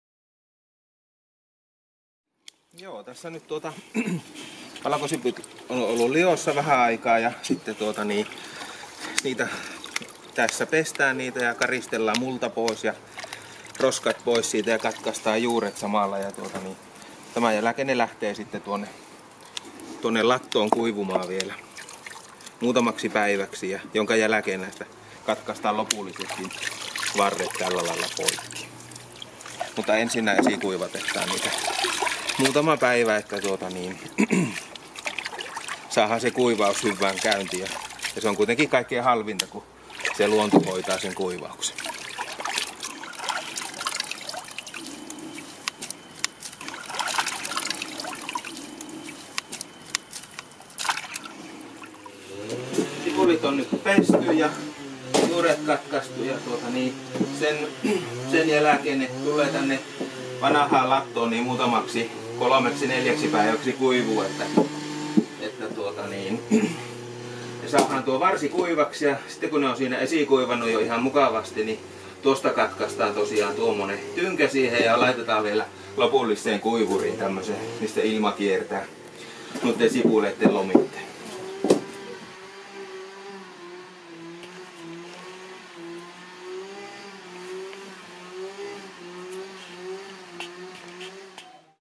Valkosipulin kuivauta Muhoksella 10.07.2020 / Kuvaaja: Sami Aspelund
Muhoksen Paroonin yrittäjä Antti Hirvonen valkosipulien putsaus- ja kuivaustöissä.